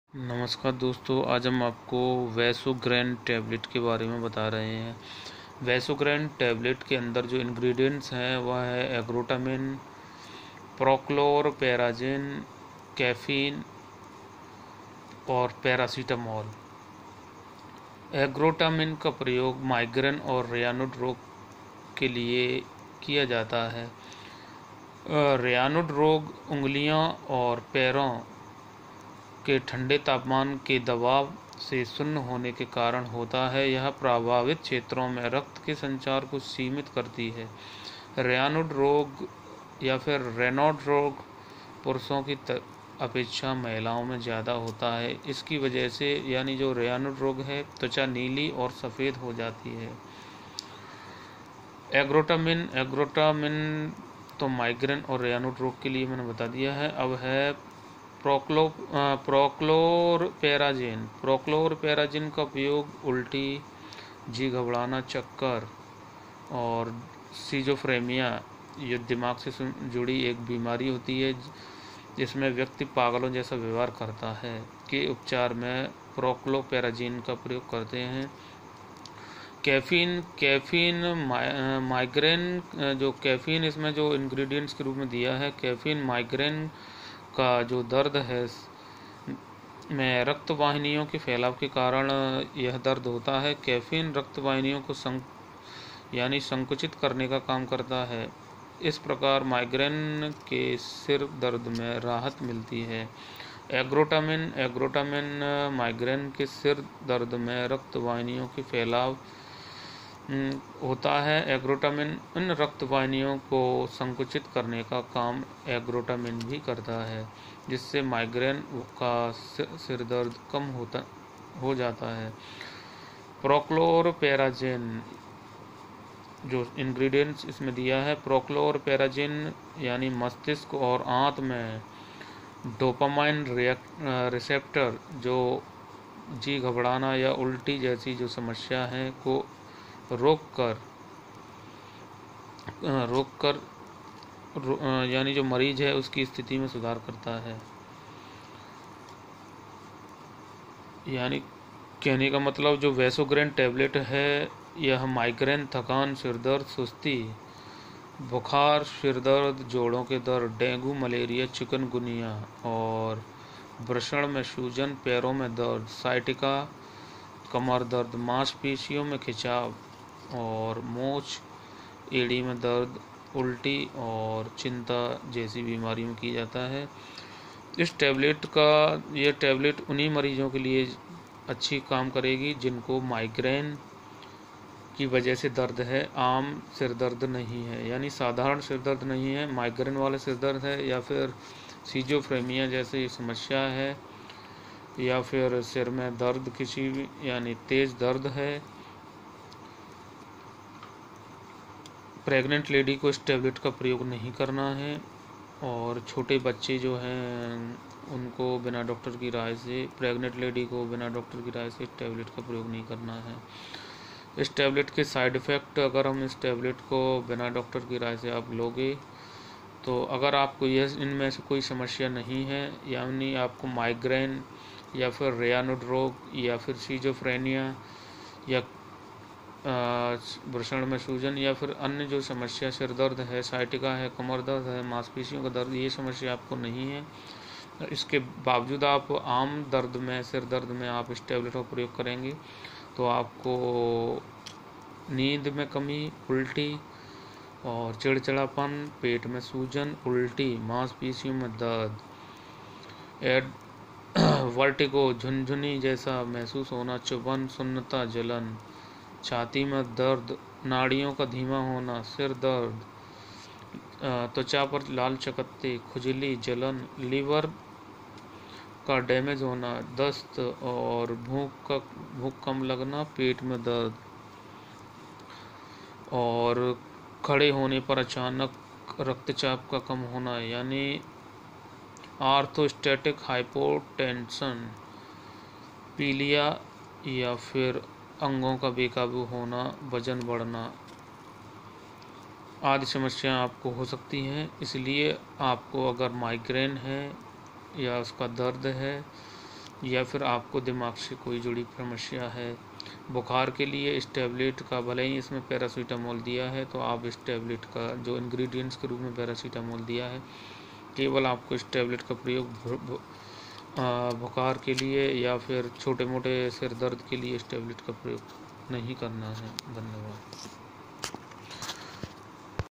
VASOGRAIN Tablet Rx _ Ergotamine , Prochlorperazine , caffeine , Paracetamol ( migraine & schizophre
Vasogran tablet
            केबल migraine और Schizophrenia , बृषण में सूजन , डेंगू , मलेरिया , chikangunai , Apnea , infantile , migraine Se Juda fever के लिये ही इस teblet का प्रयोग करना चाहिये ।
           Pregnant lady , छोटे बच्चे को इस टैबलेट का प्रयोग बिना doctor की राय से नहीं करना चाहिये ।
 Vasogran tablet की market price 72 Rs. है।
   14 tablet होती हैं एक पत्ते में ।